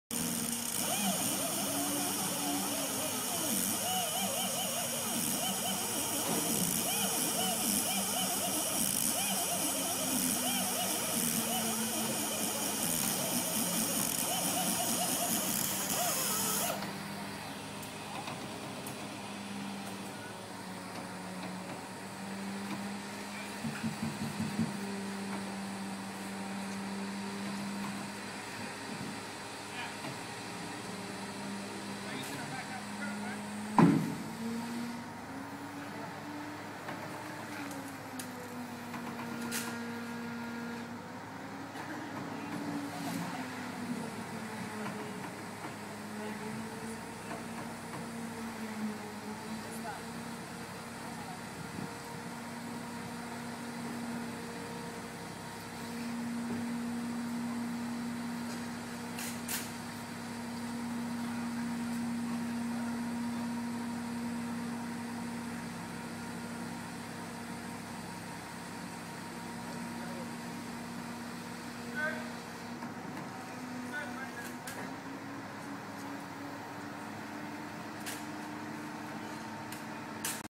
The Boomerang Boat getting ready to be loaded for transport to Washington DC
Skipper Liner getting ready to be loaded onto transport trailer for the trip to her new home in Washington DC after a complete restore by Gulf Coast Complete Marine in Kemah, TX. She will be sailing on the Patomic with her new owners The Boomerang Boat.